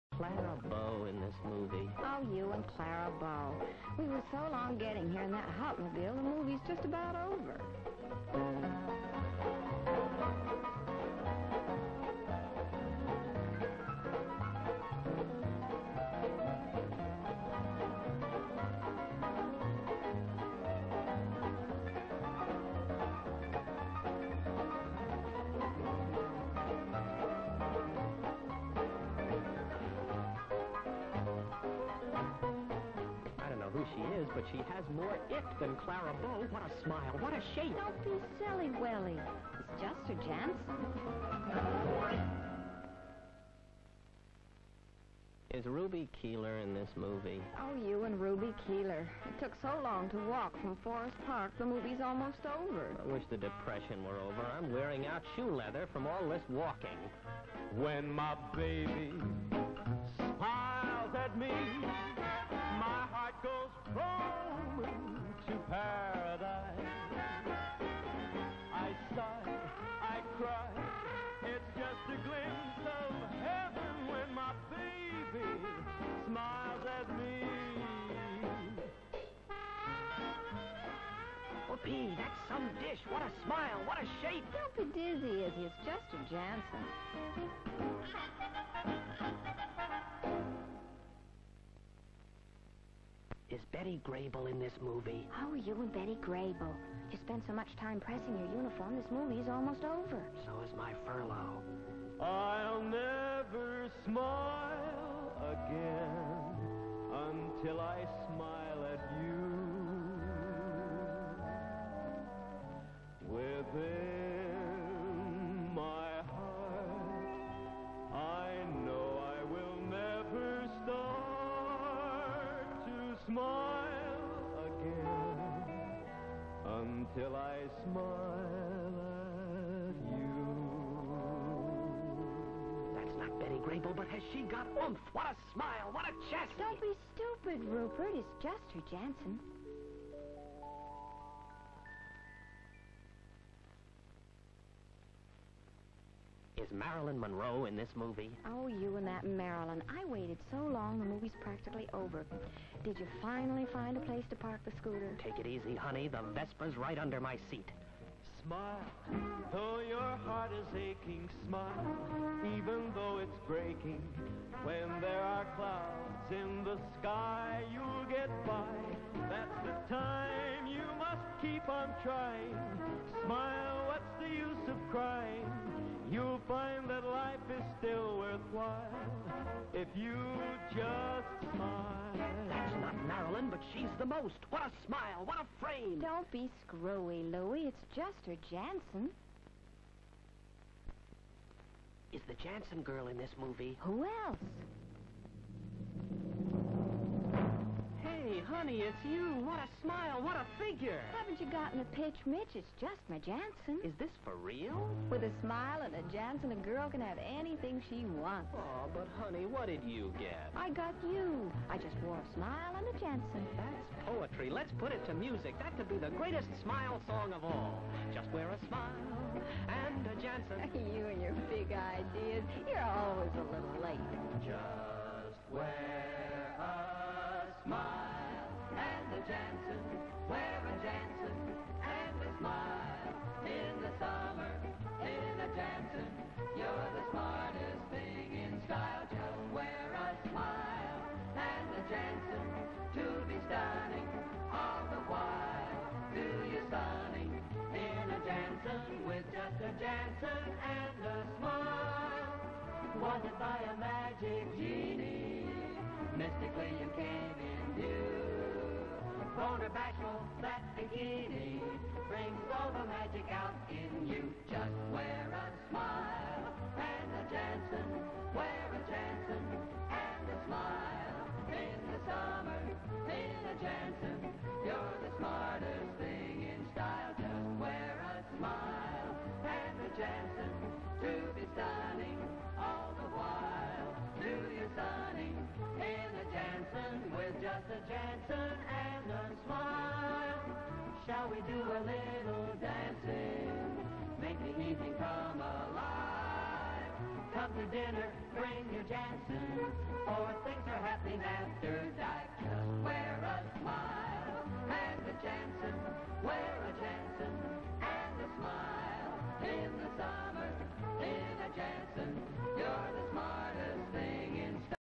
Jantzen 1910 to 1960, 1960's -- Film 17
Jantzen 1910 to 1960.  Series of animated stills of woman dancing in 20th century fashions from 1920s onwards.  Advertising for Jantzen clothes.